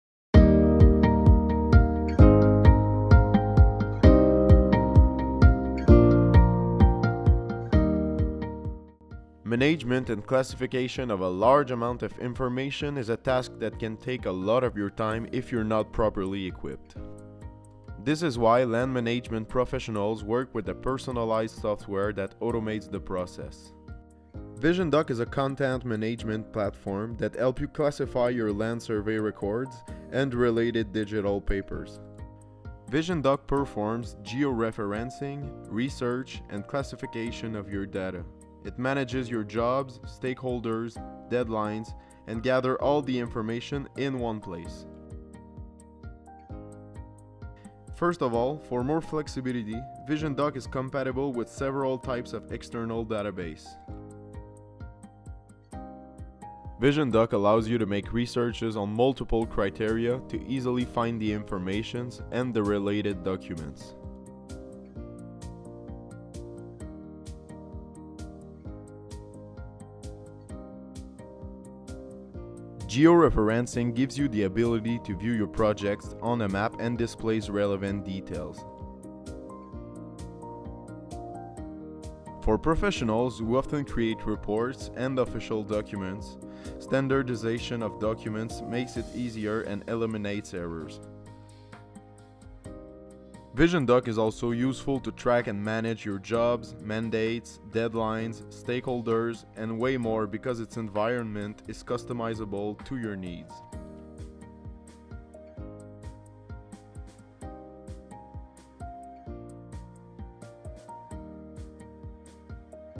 Discover the most effective way to manage your land survey records
Vision Doc is a document management software that performs the classification of your records and automates the process. This platform allows us to effectively manage land survey records and any information related to the land management.

For any land management professionals such as certified appraisers, mining companies, governmental bodies, municipalities, civil engineers, and land surveyors.
Try this land document software which is the most customizable on the market

-Georeference Your Jobs
-Powerful Search Engine
-Automatic documents filling
-Activities Management
-File Referencing
-Outlook Compatible